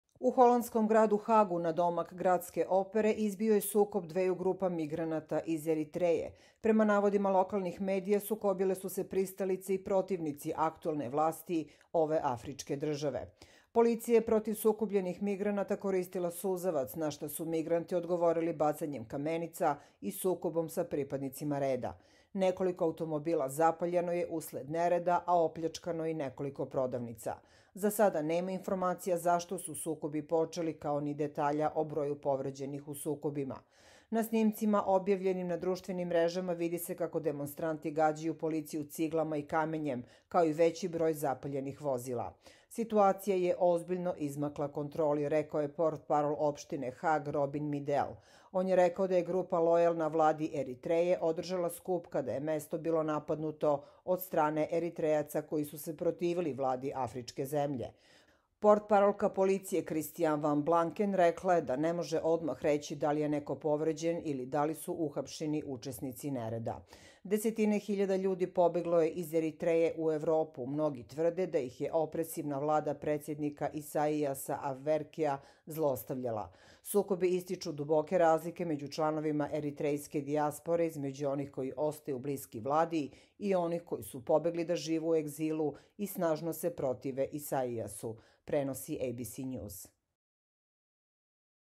Haos u Holandiji: Žestoki sukobi migranata i policije
U holandskom gradu Hagu, nadomak gradske opere, izbio je sukob dveju grupa migranata iz Eritreje. Prema navodima lokalnih medija, sukobile su se pristalice i protivnici aktuelne vlasti ove afričke države.